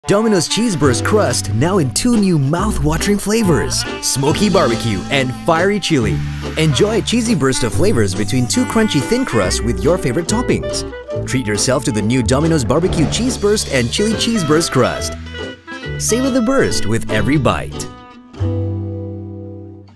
Domino's Malaysia Cheese Burst Crust (English TVC)
Try our NEW Chili Cheese Burst Crusts and BBQ Cheese Burst Crusts today and give your tastebuds a burst of flavour for the same surcharge!